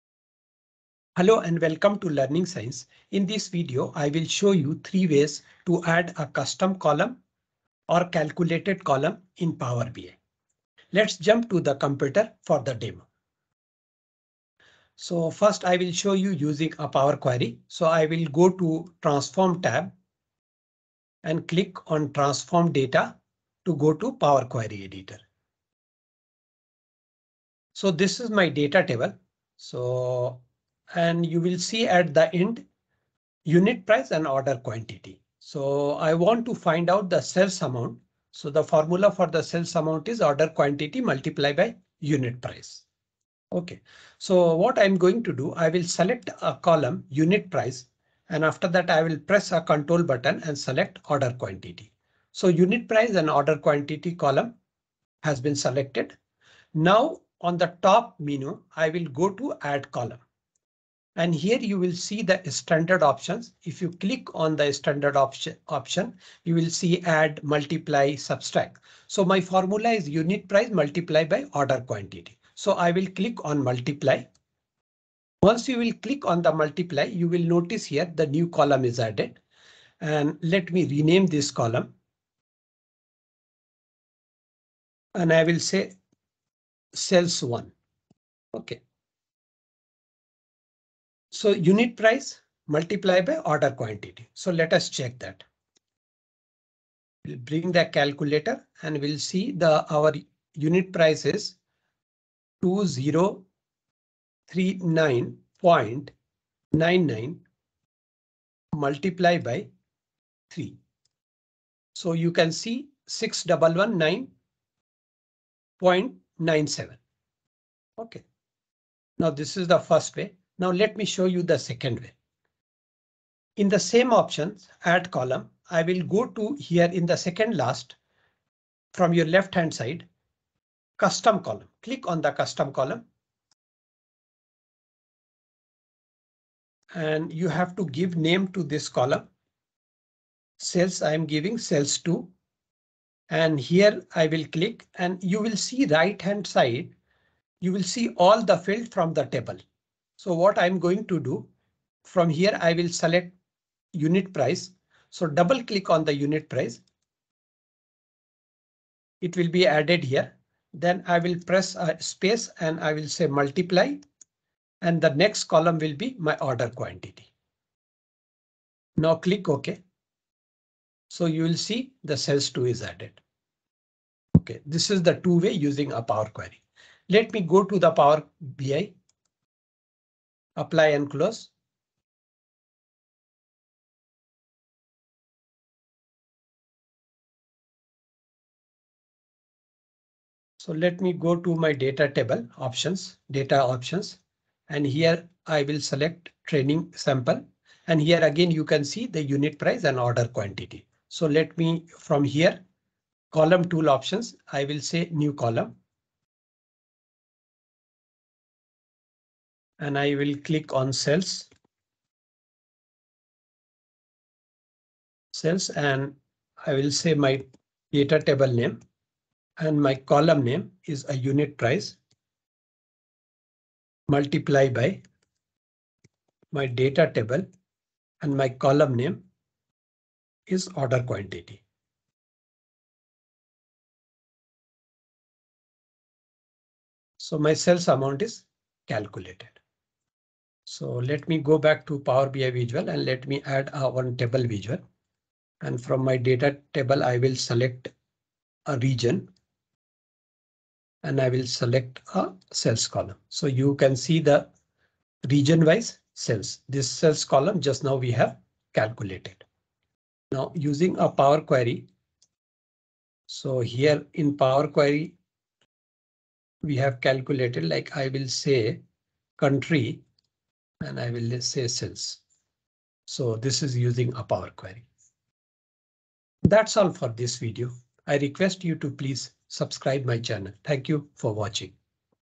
3 ways to add calculated or custom column in Power Bi
Explained : How to add custom column and calculated column in Power BI using DAX and Power Query.